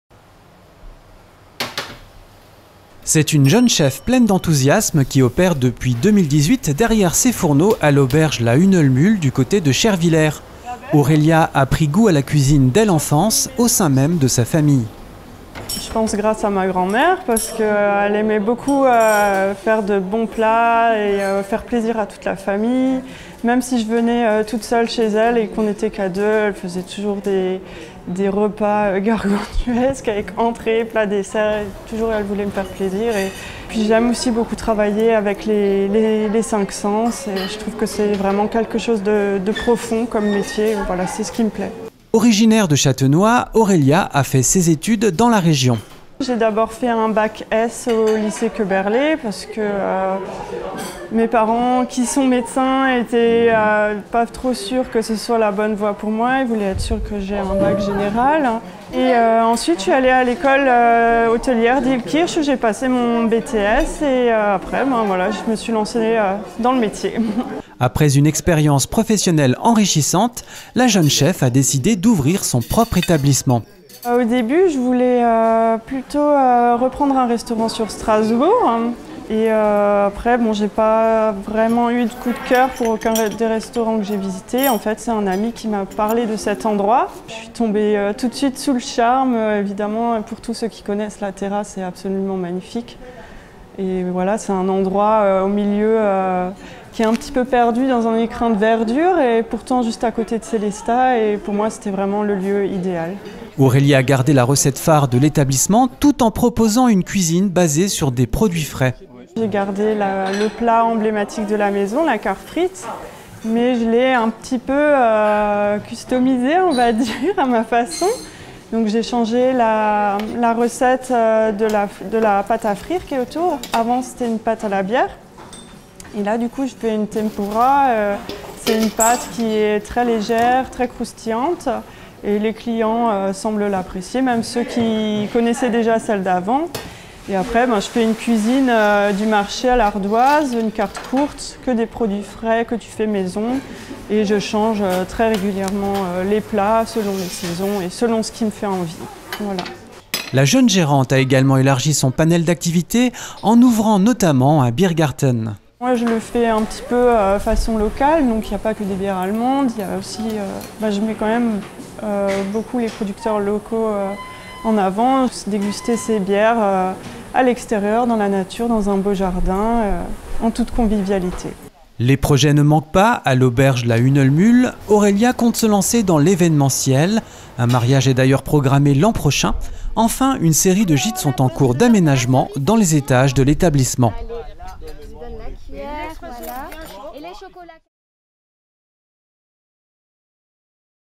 Aurélia, jeune chef à l'Auberge de la Hühnelmühle à Scherwiller
Entretien avec Aurélia Grall, une jeune chef à l'Auberge de la Hühnelmühle dans la Commune de Scherwiller !